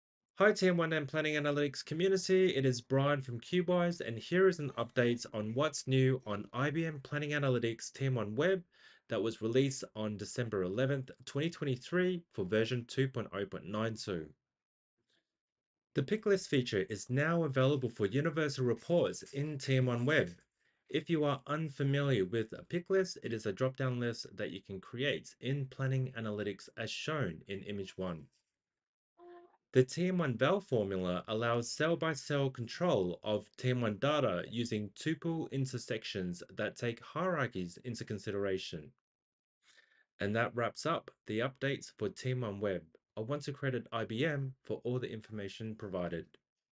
IBM Planning Analytics TM1 Web 2.0.92. Released on Dec 11, 2023.
What's New for IBM Planning Analytics TM1 Web.

Key Takeaways
- Pick List Feature for Universal Reports: Enhance your reporting with seamless drop-down lists.
- TM1Val Formula Unleashed: Take precise control with cell-by-cell management of TM1 data.